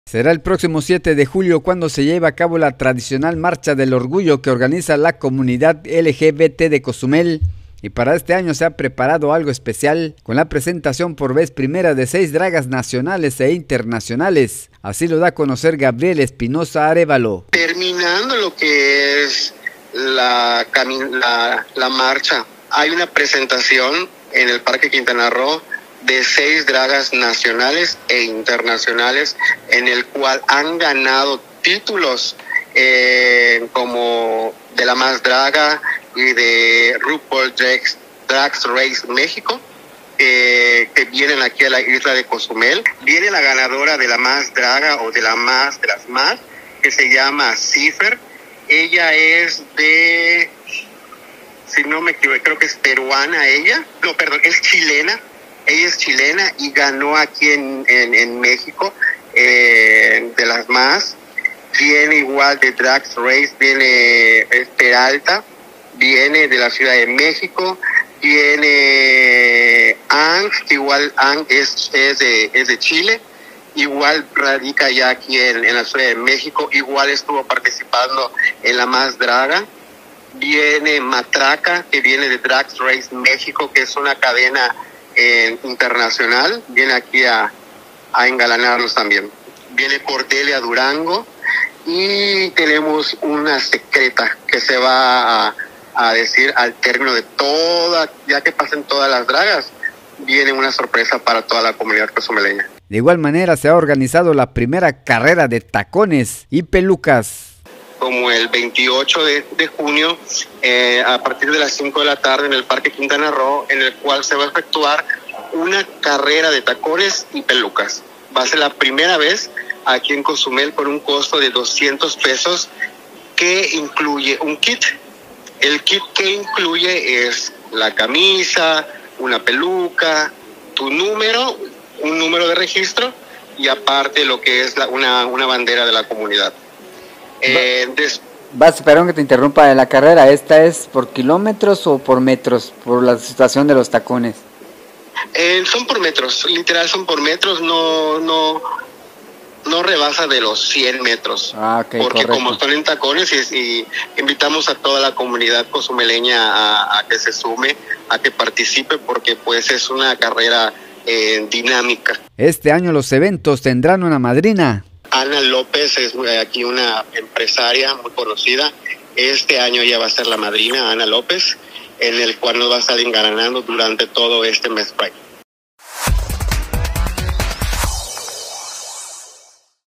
Preparan la marcha del orgullo que organiza la comunidad LGBT de Cozumel
Preparan la marcha del orgullo que organiza la comunidad LGBT de Cozumel que este año incluye la presentación de seis dragas Nacionales e Internacionales.